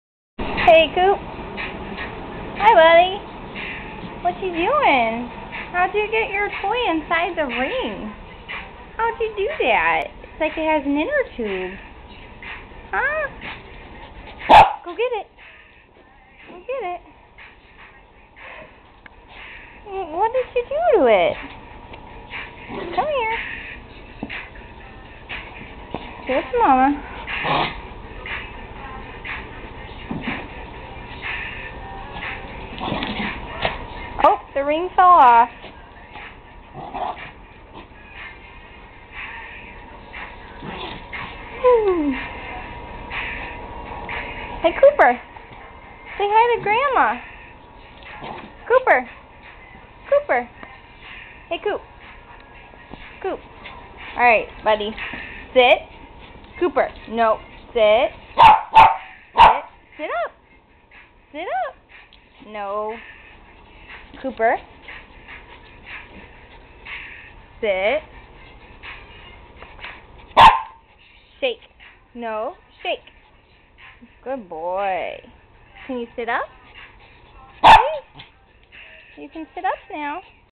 Toy mix-up
Cooper playing--he somehow figured out how to get his bear thing to wear his ring as an intertube.